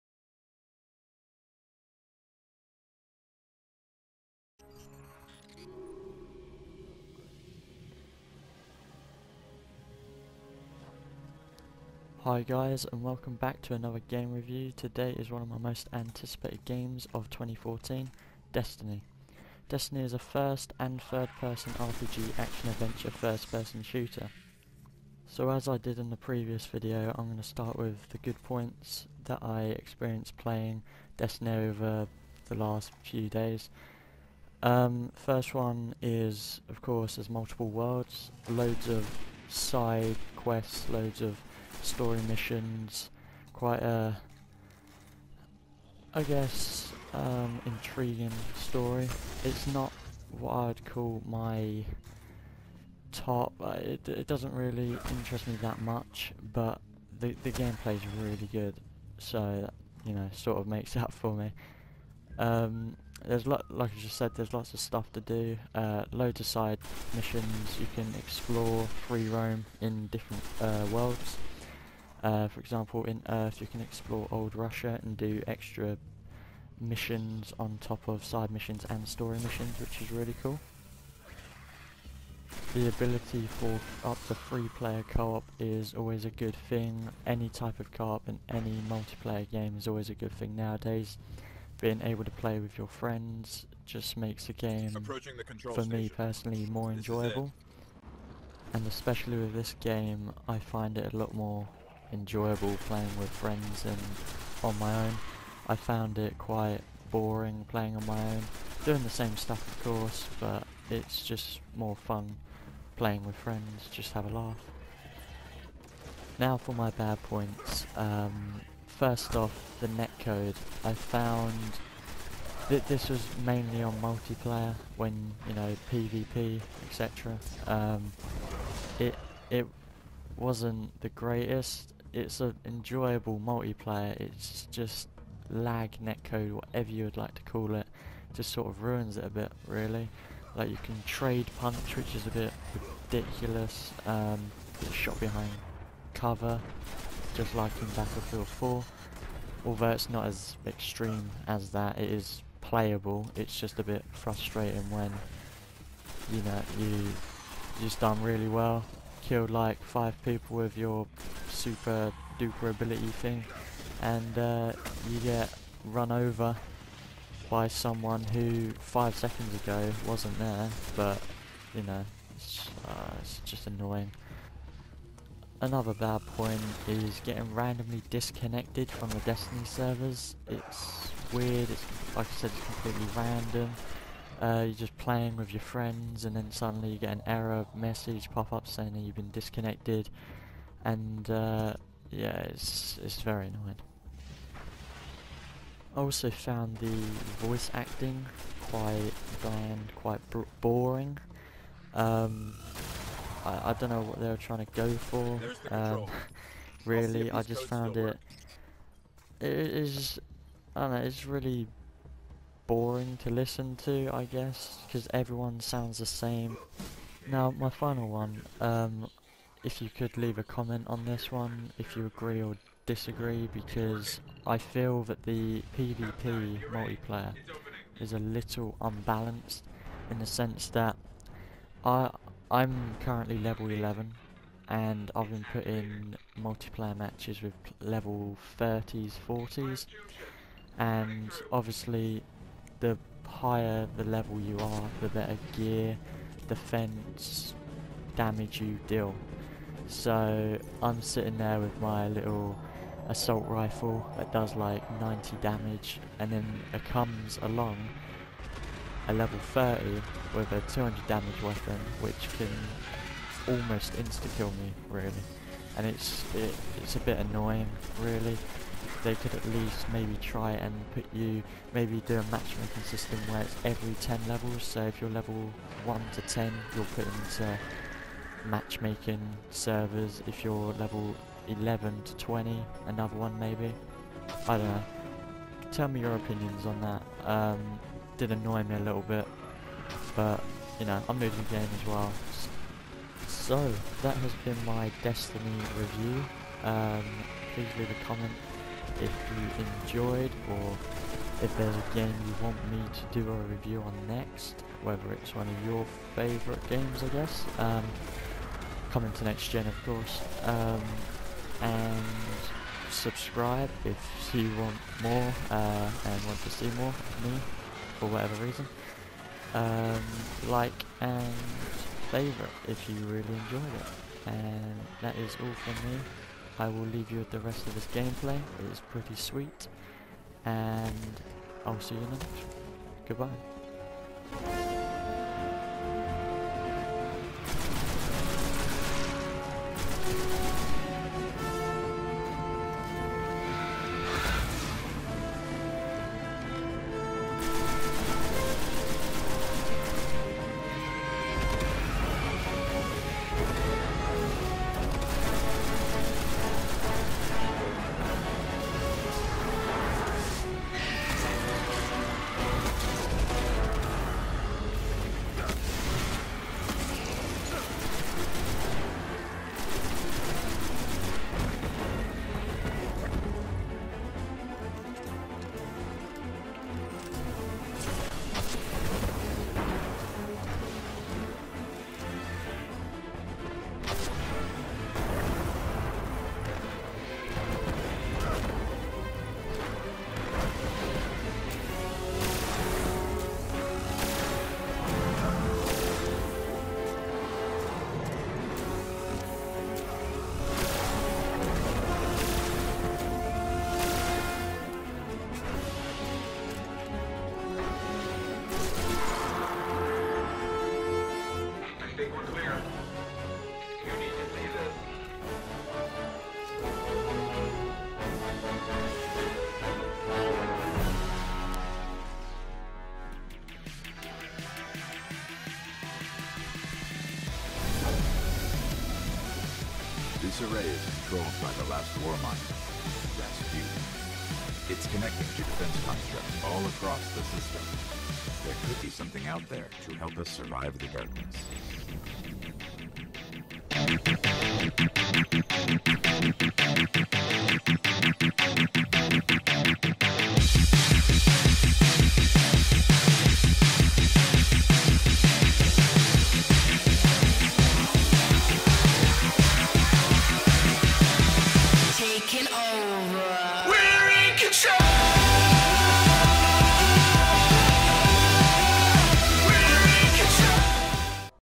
GAME REVIEW - Destiny
Hi guys here is my game review for Destiny. In this video I review this weeks release 'Destiny' (Released 9th September 2014) where I talk about the good points and the bad ones. :)

Buy This Game:

XBOX ONE

Twitch - I sometimes stream here check it out :D